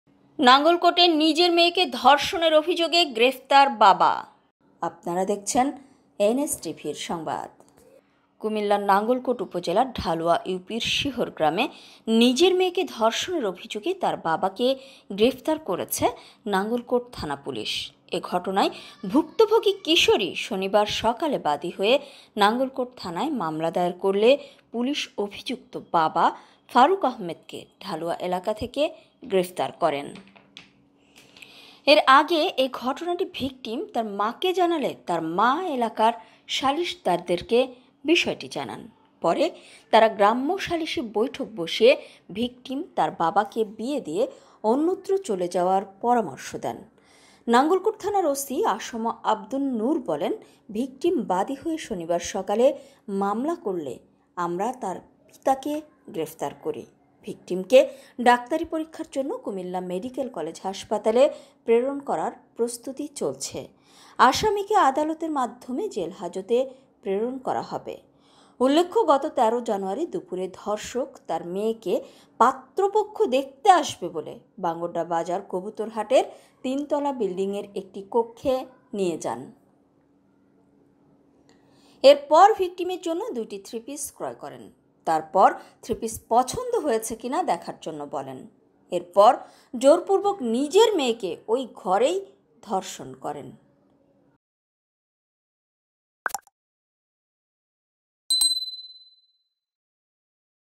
নিজের মে_য়েকে ধ*র্ষনের অভি*যোগে বাবা গ্রে_প্তার NSTV
Father arrested for raping his daughter in Dhalua area of ​​Nangalkot upazila of Comilla